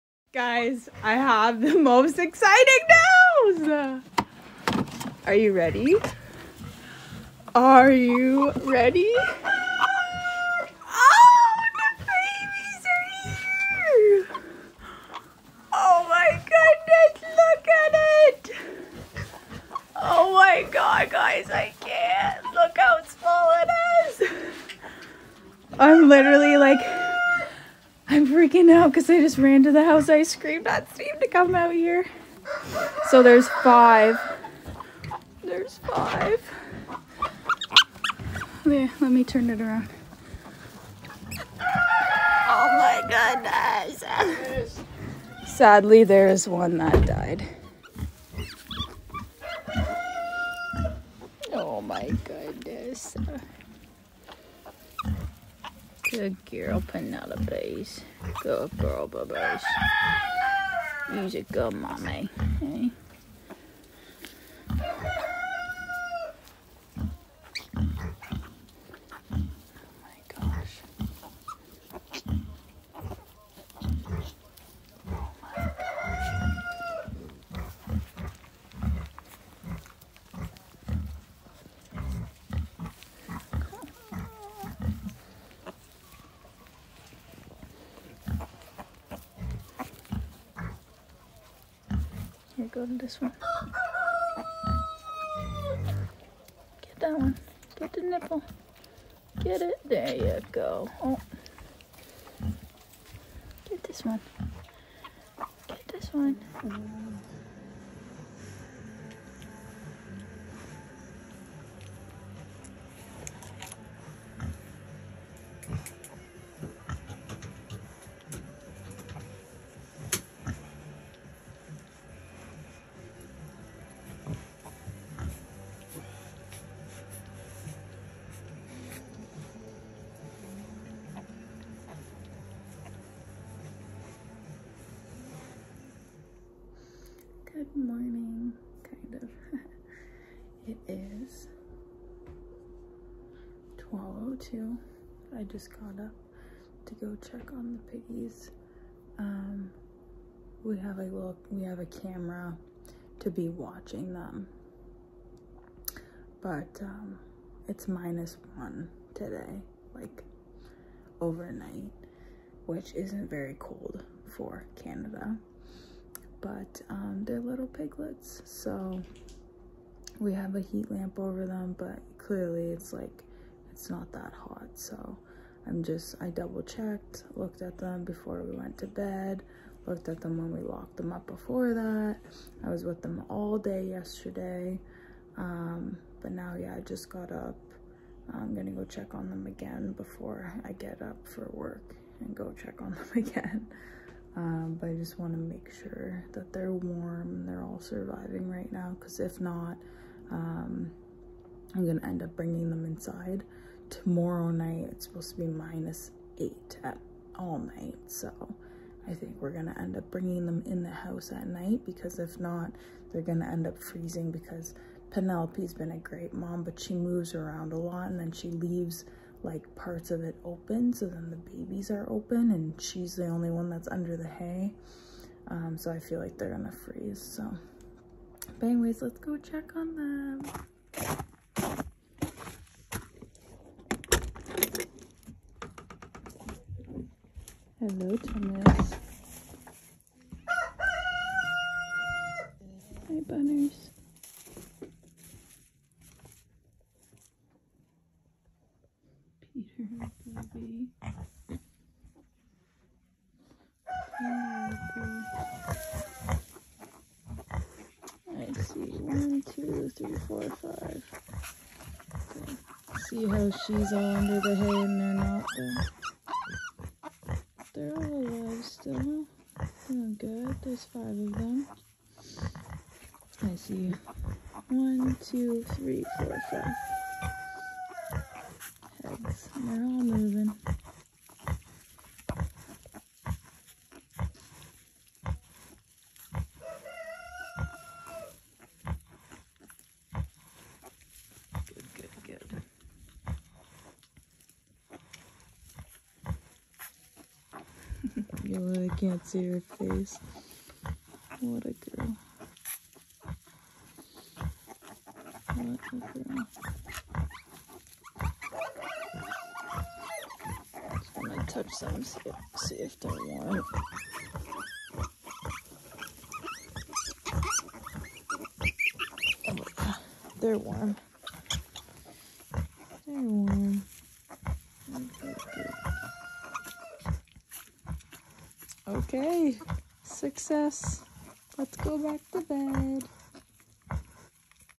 First 24 hours with potbelly piglets!! 5 new babies on the homestead!!
Penelope had 5 babies!! Howwww cuteeee!!!
Christmas piglets! haha